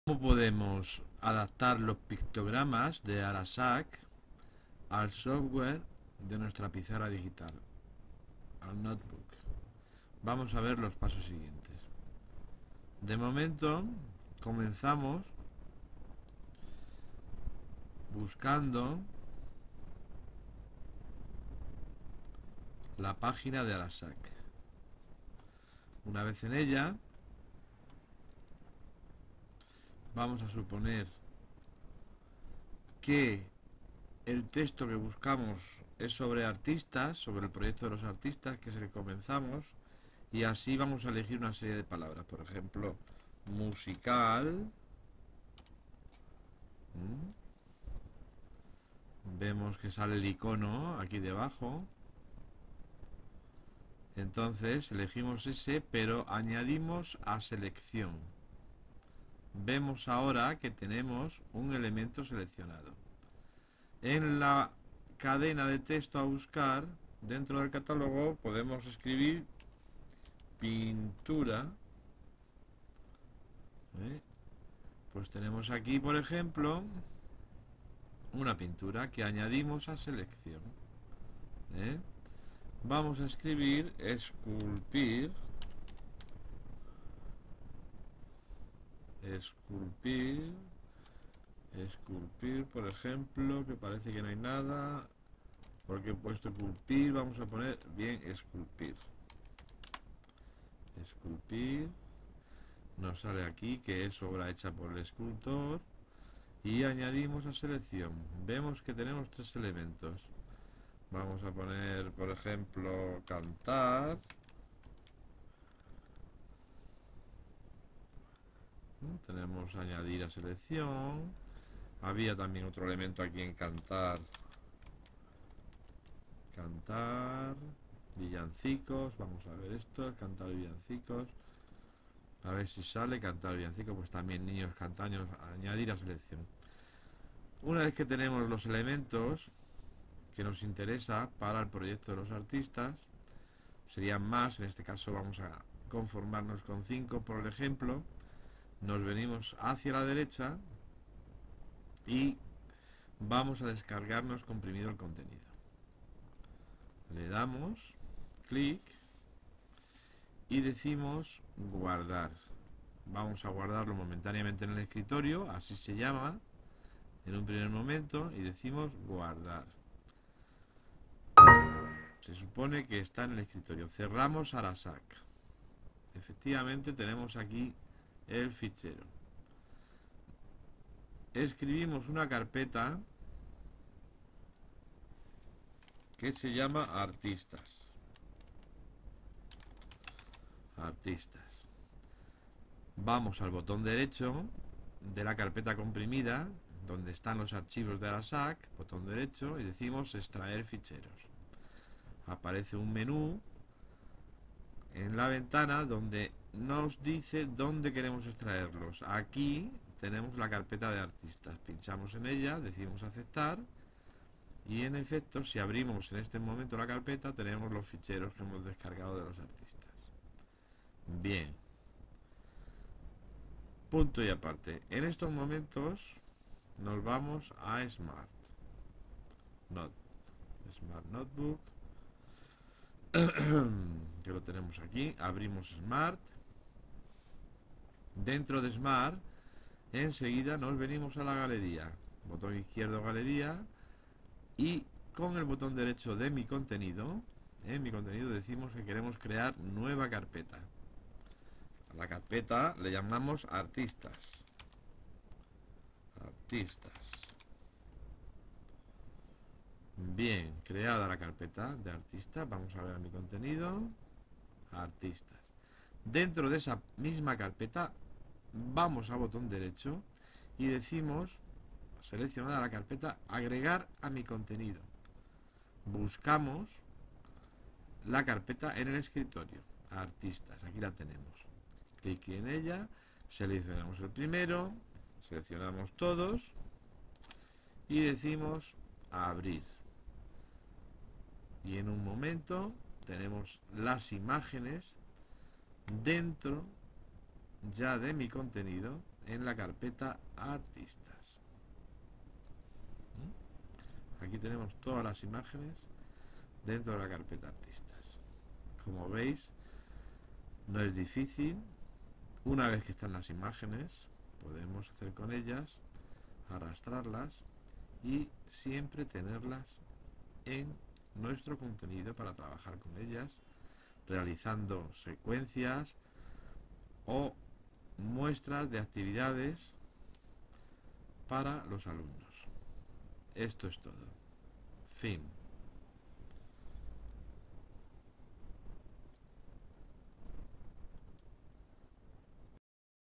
Proyectos Innovación 2011-2012: Recursos TIC para alumnado con TEA
En el proyecto de "Recursos TIC para alumnado con TEA" se ha generado una unión del programa de pizarras digitales smart y los pictogramas de ARASAAC que se usan a diario.

Este vídeo es un tutorial para la web de ARASAAC que explica cómo se pueden adaptar los pictogramas de ARASAAC al software de nuestra pizarra digital